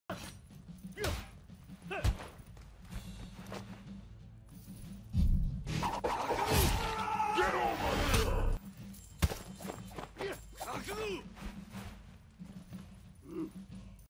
Shinobi is Scorpion
I really like the new character and I found out why I love this character so much. He reminds me of scorpion! Watch and see for yourself!